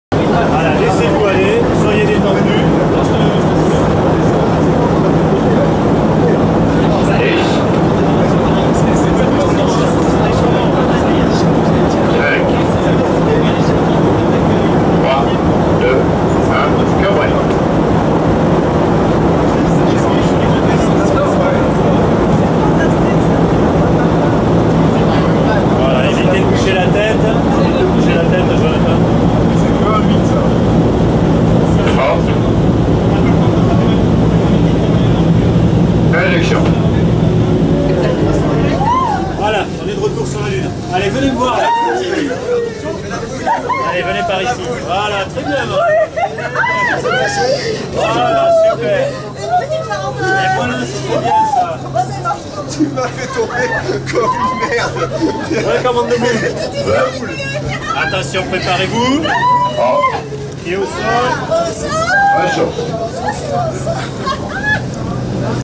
AIR ZERO-G - PARABOLE LUNAIRE - N°3 - 15/03/13
Une vidéo brute de la parabole N°3 (lunaire) du vol Air Zero-G CNES NOVESPACE du 15 mars 2013. Images HD3D Ewen Chardronnet